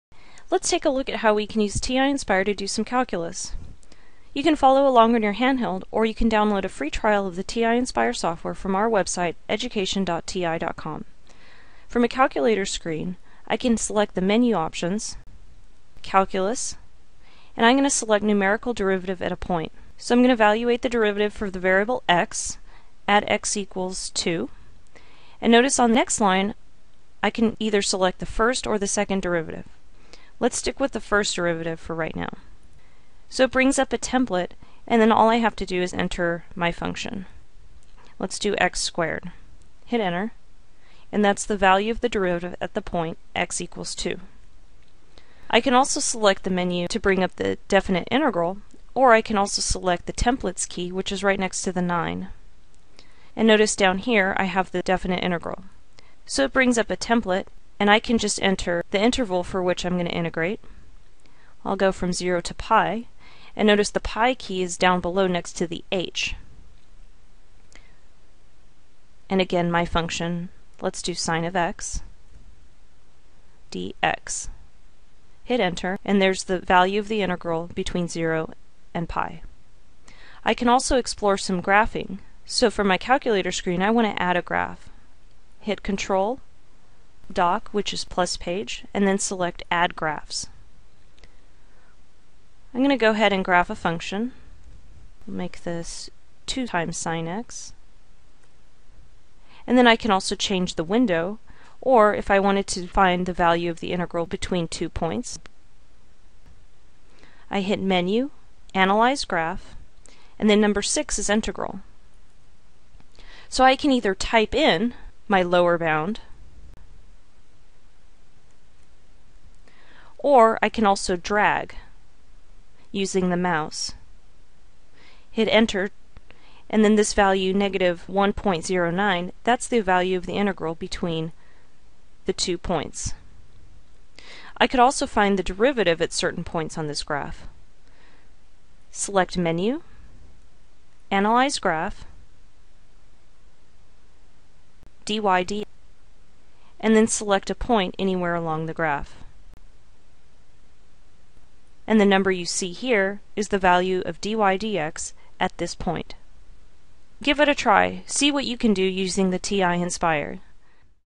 TI-Nspire CX Handheld: TI-Nspire for Calculus
Use the TI-Nspire CX graphing calculator or software to find numerical derivatives and definite integrals using the calculator and graph options.

To try it out for yourself, download a free 90-day trial of TI-Nspire Teacher Software